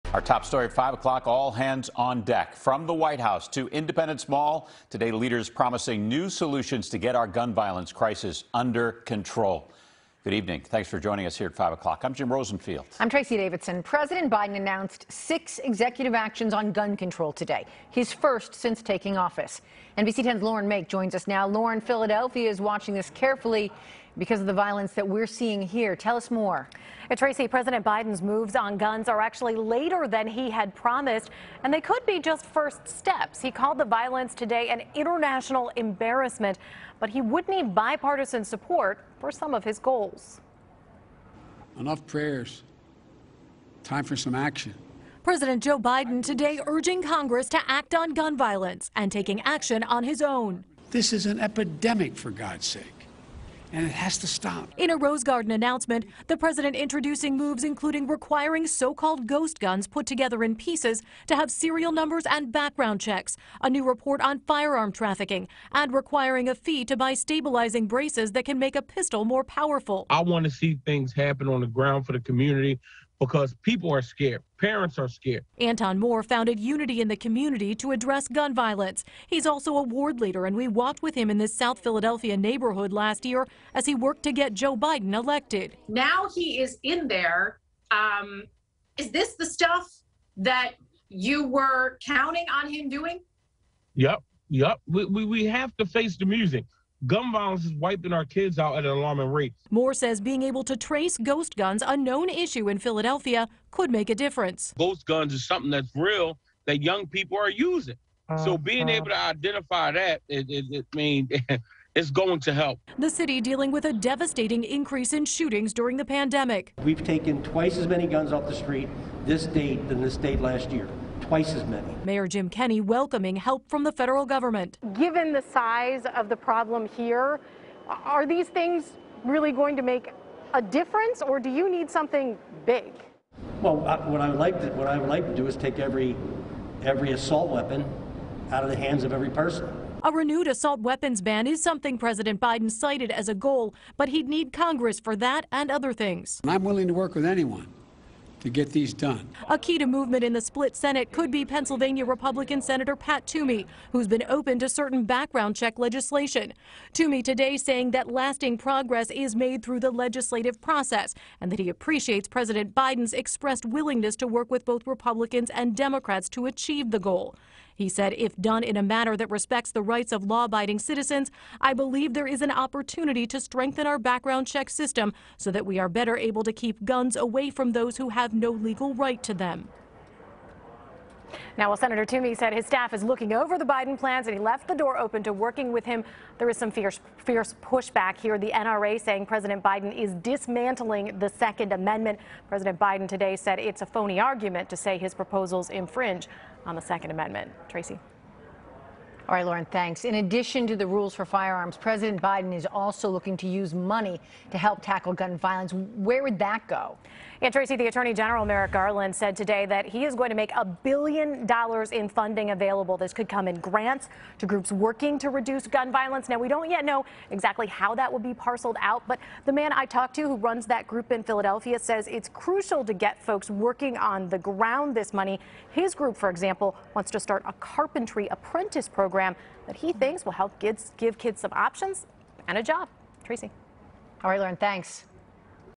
President Biden Takes First Actions Against Philadelphia Gun Violence | NBC10 Philadelphia
The president announced six executive actions on Thursday to help Philadelphia’s ongoing gun violence crisis. NBC10’s Lauren Mayk has the details.